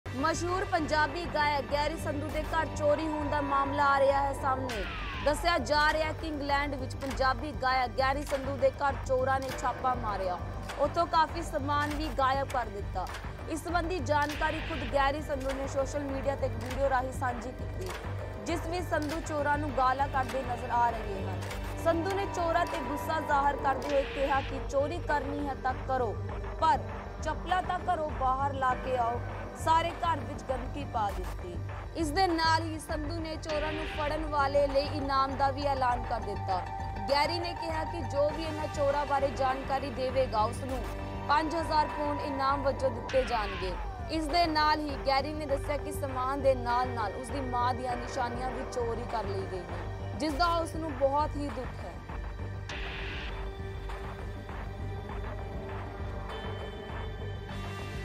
ਮਸ਼ਹੂਰ ਪੰਜਾਬੀ ਗਾਇਕ ਗੈਰੀ ਸੰਧੂ ਦੇ ਘਰ ਚੋਰੀ ਹੋਣ ਦਾ ਮਾਮਲਾ ਸਾਹਮਣੇ ਆਇਆ ਹੈ।
ਇੰਗਲੈਂਡ 'ਚ ਪੰਜਾਬੀ ਗਾਇਕ ਗੈਰੀ ਸੰਧੂ ਦੇ ਘਰ ਚੋਰਾਂ ਨੇ ਛਾਪਾ ਮਾਰਿਆ ਹੈ ਅਤੇ ਉਥੋਂ ਕਾਫੀ ਸਾਮਾਨ ਗਾਇਬ ਹੋ ਗਿਆ ਹੈ।